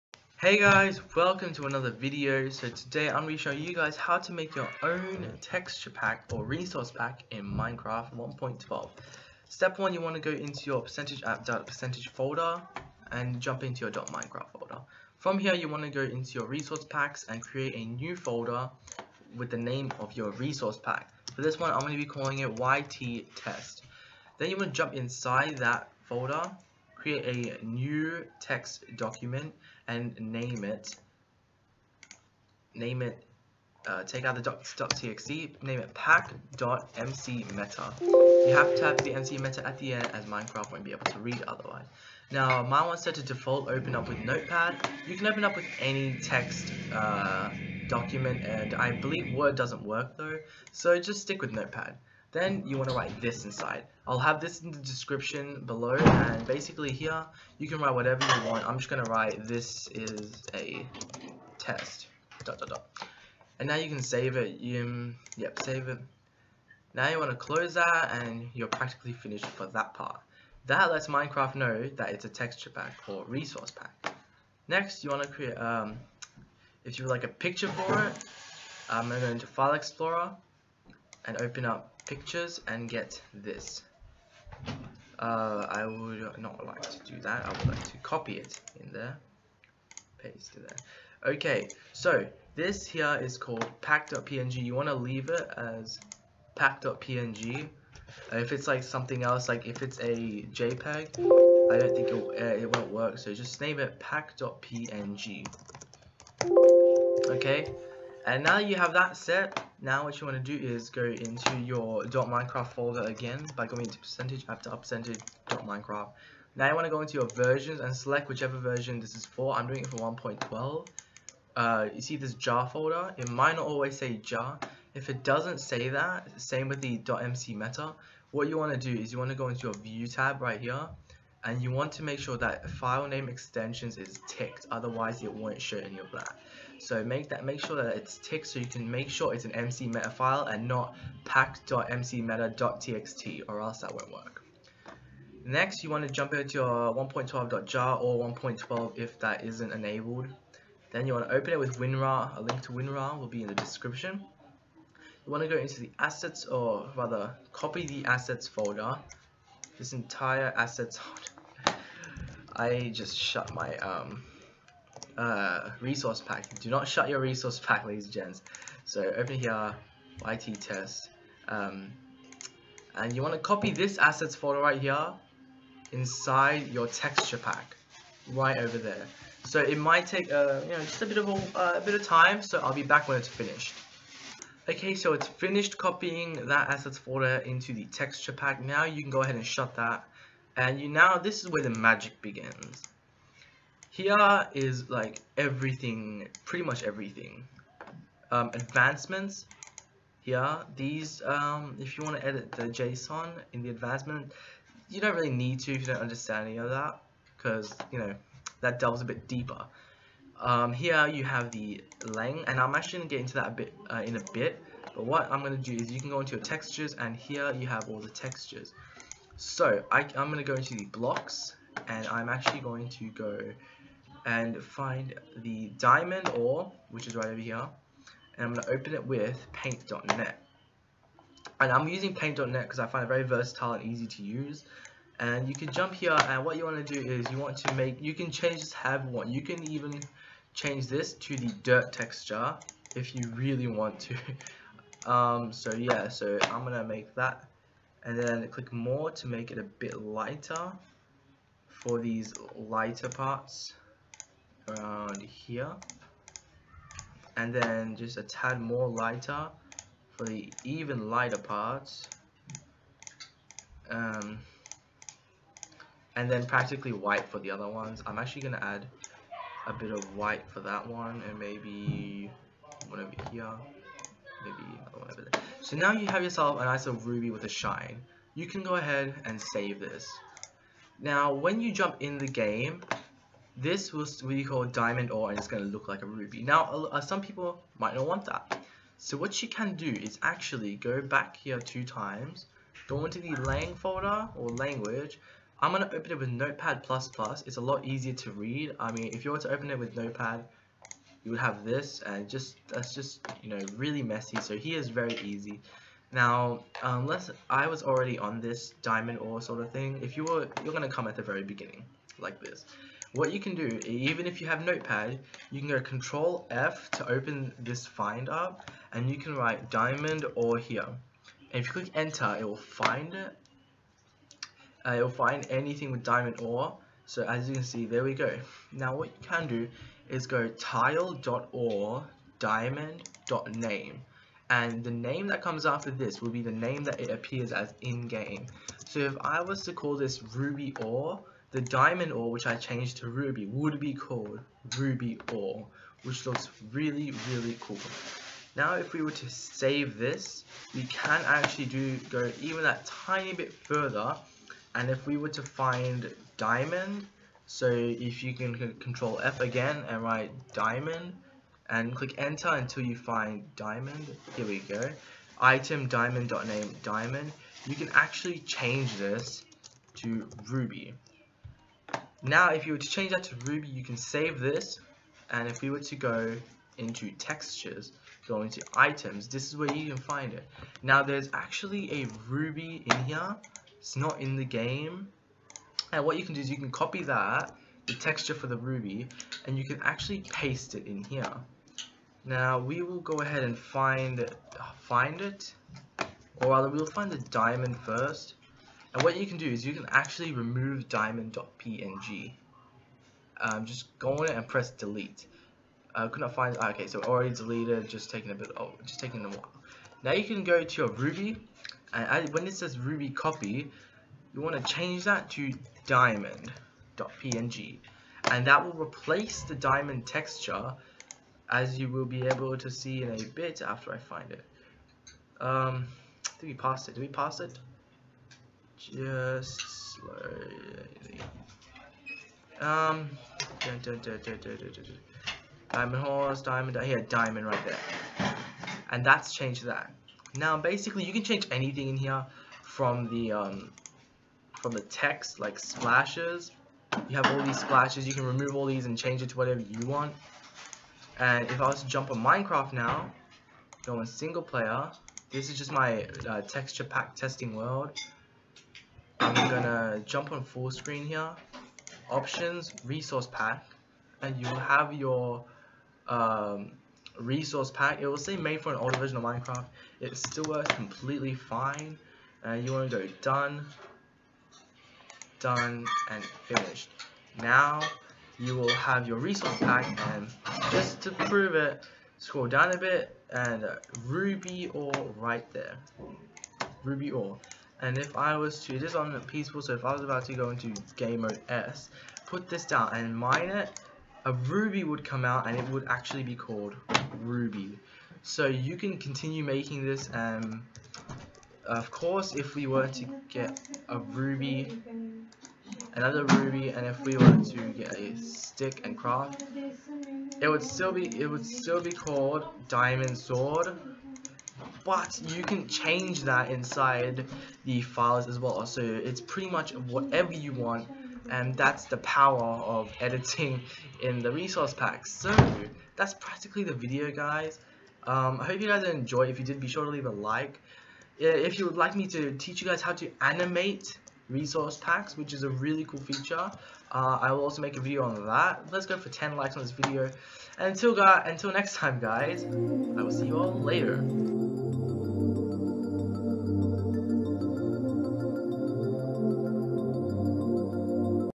How to make your own resource pack in minecraft 1.12
Today I show you how to make and completely customize your own Minecraft 1.12 texture/resource  pack, as always... I hope you enjoy!

CODE:
 "pack" : {
   "pack_format" : 3,
   "description" : "DESCRIPTION" } }